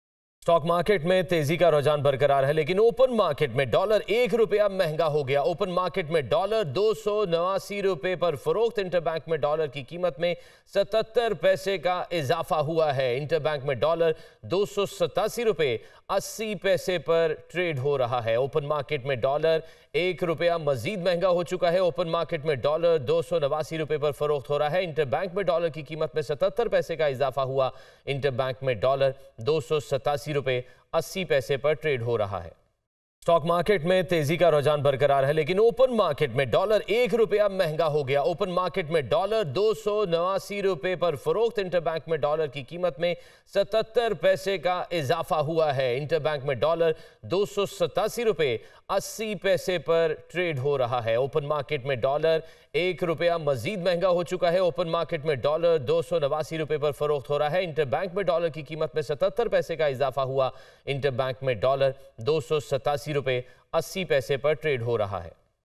Dollar Price Increased | Dollar Rate Updates | Dunya News
The US dollar started the new business week on Monday from where it had left during the last session, with a Rs1 gain in open market exchange rate, as the rupee seems unable to halt the latest slide which is erasing the gains it had made after reaching a record low against the world’s dominant currency.

As the market approached the midday trading, the dollar was available for Rs289 amid a high demand, which is also being attributed to market manipulation, as the investors are opting for the safe haven currency amid the ongoing International Monetary Fund (IMF) of the $3 billion stand-by arrangement.

On the other hand, the dollar’s official exchange rate in interbank intraday trading had so far increased by 87 paisa, which means the greenback was available for Rs287.90 against the previous closing of Rs287.03 on last Friday.

The weakening rupee coincides with the history-making bullish trend being witnessed in the stock exchange where the KSE-100 has now shattered another record by crossing the 56,000 ceiling.

Last month, the State Bank of Pakistan’s Monetary Policy Committee had decided to maintain the interest rates to the current record-high level of 22pc despite the signs of an easing inflation, making many in the market to expect a rate cut.

The rupee is under pressure amid a high demand – described by many as a return of market speculation after the short-lived government crackdown – as the IMF is pressing ahead with its demand to meet all the conditions required under the $3 billion stand-by arrangement.

Hence, this scenario is creating uncertainty as experts believe that even the release of the second tranche after a successful first review won’t improve dollar supply.

The IMF has been adamant to stick with the criterion of market determining the currency exchange rate, thus closing the door for any government intervention to boost the local currency.